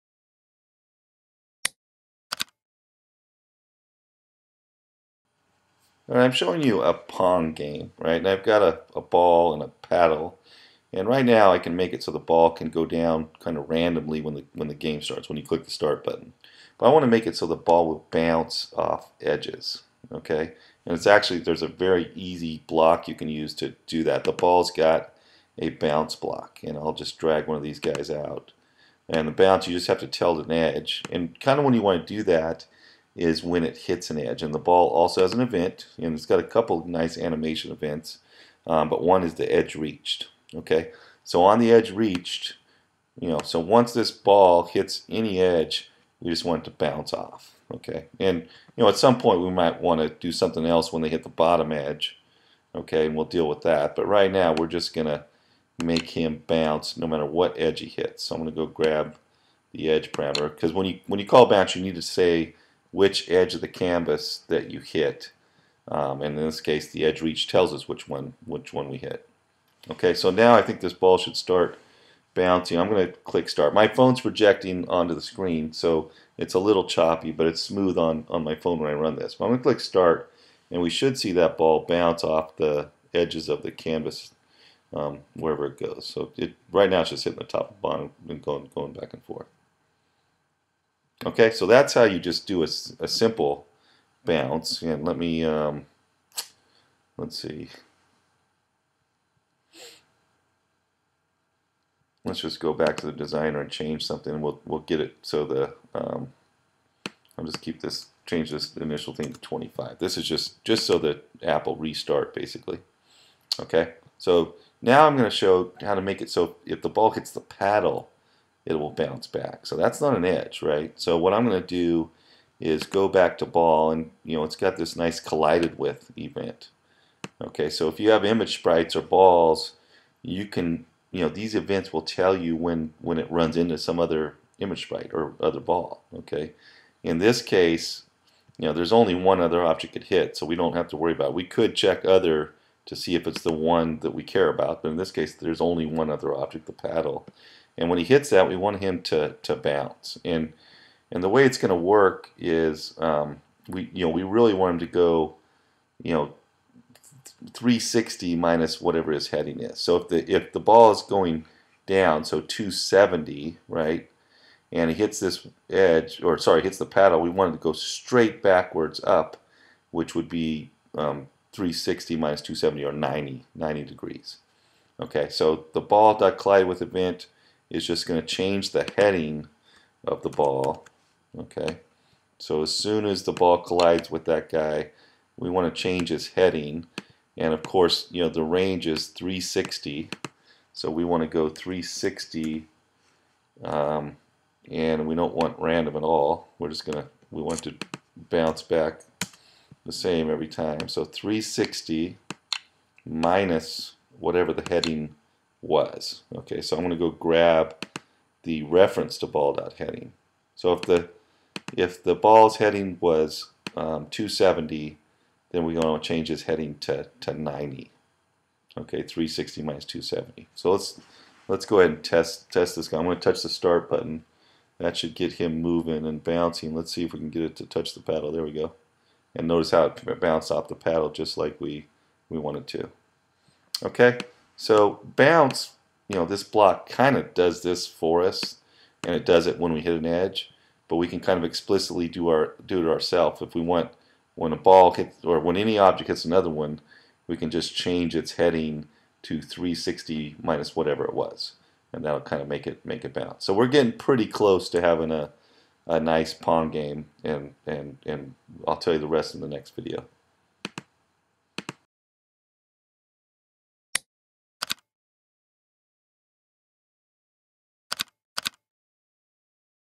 Code "Pong" with App Inventor 2 (p3)
In this appinventor.org screencast series, Professor Wolber demonstrates how to build the classic game "Pong" with App Inventor 2.